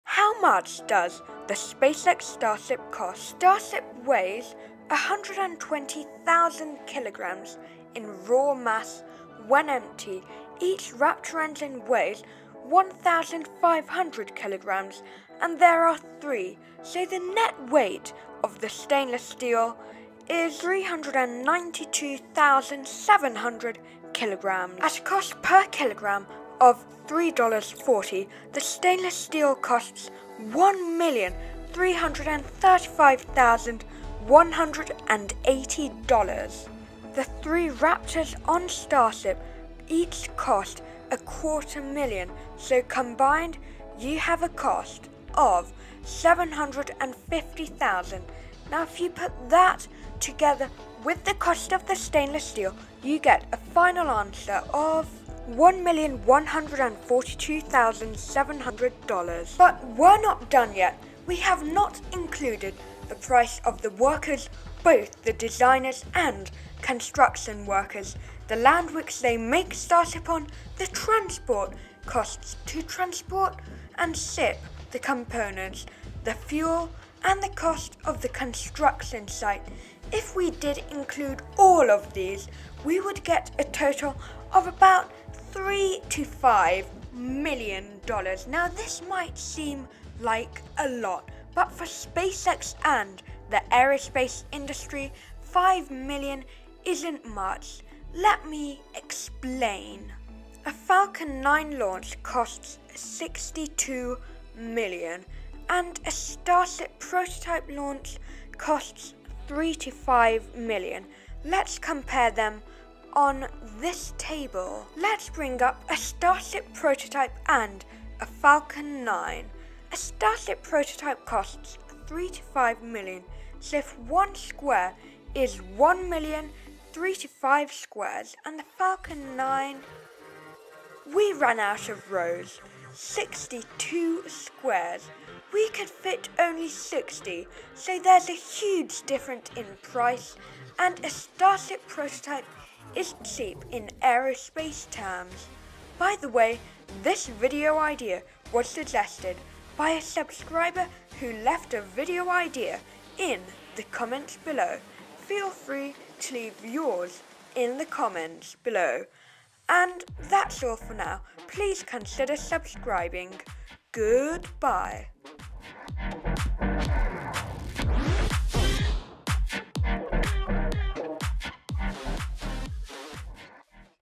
how EXPENSIVE is starship
in this video i will tell you how much a spacex starship prototype ACTUALLY costs, we will break down the cost of the raptor engines, the price of the steel and many more factors, before comparing the price to other rockets such as the spacex falcon 9!!!

TIME STAMPS:
00:00 - raw material costs
01:00 - work and other factors
01:20 - final price
01:29 - price comparison with falcon 9
02:27 - how to suggest a video idea
02:35 - outro

BTW: thank you so very much for 230 subscribers, it means a lot + we now have a custom channel ulr!